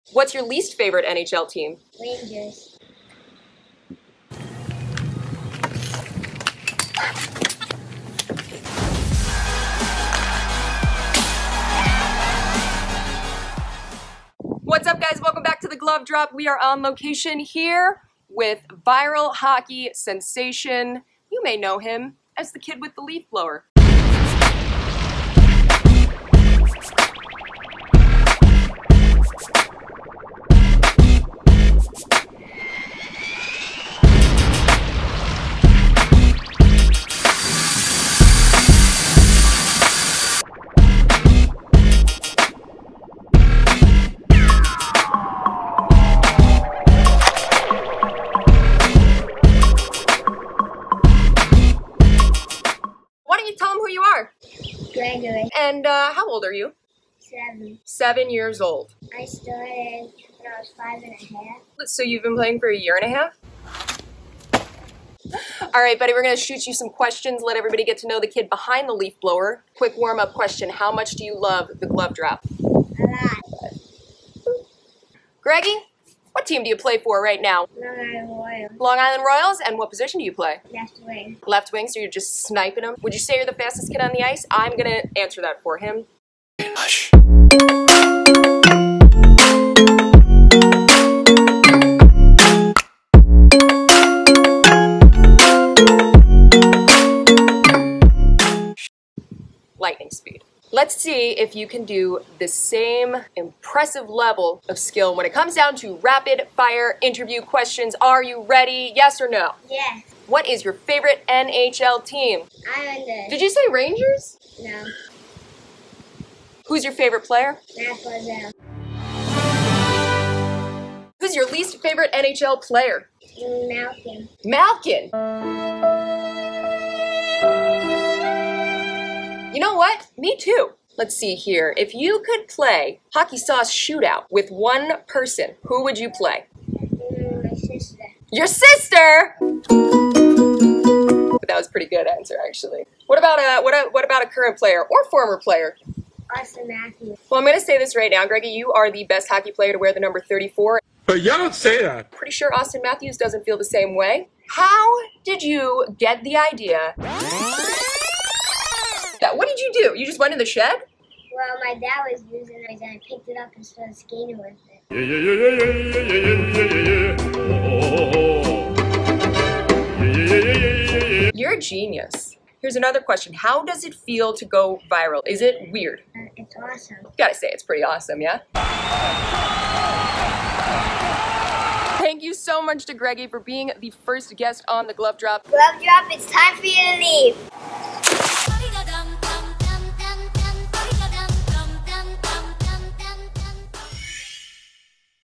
Interview with @GregoryHockey2012 who went Viral Skating with a Leaf Blower!
7 year old Long Island hockey stand-out, Gregory Mast, has gone viral after he paired impressive skating ability with a leaf blower and took to the streets.
Check out the interview here and be sure to follow him on IG @GregoryHockey2012 to see more mind blowing hockey skills!

Check out Long Island Hockey Co. and get your hands on the sickest gear to rep L.I.!